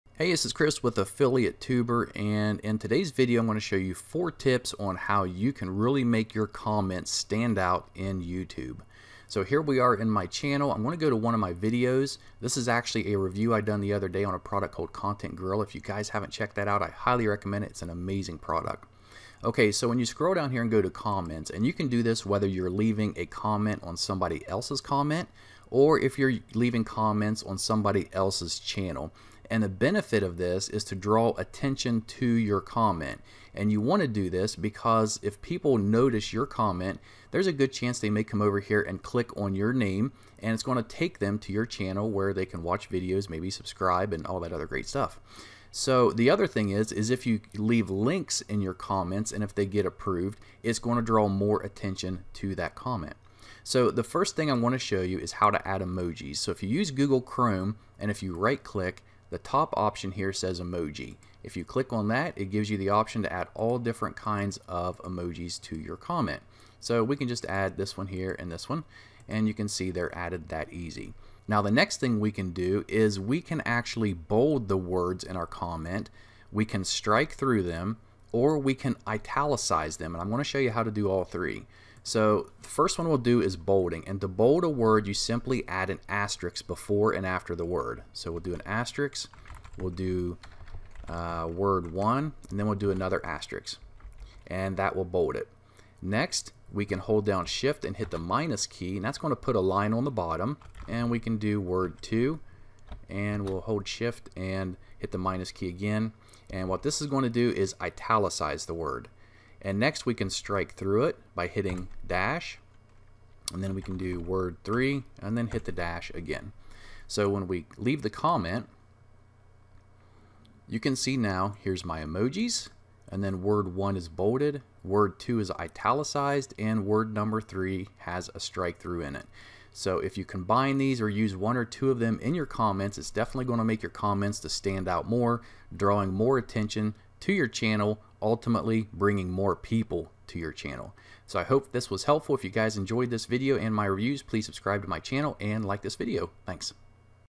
How To Bold, Italicize, Emoji & Strike-Through YouTube Comments [4 Tips]
Learn how to bold, italicize, strike-through and add emojis to your YouTube comments to make them stand out.  In this video tutorial, I'll show you how easy it is to do all 4.

Enhancing your comments with these tips can make your YouTube comments really "pop-out" especially when adding emojis to YouTube comments, drawing more attention to them resulting in viewers clicking on your name and visiting your channel.

This video will show you 4 tips on:

How to bold YouTube comments
How to italicize YouTube comments
How to strike-through YouTube comments
How to add emojis to YouTube comments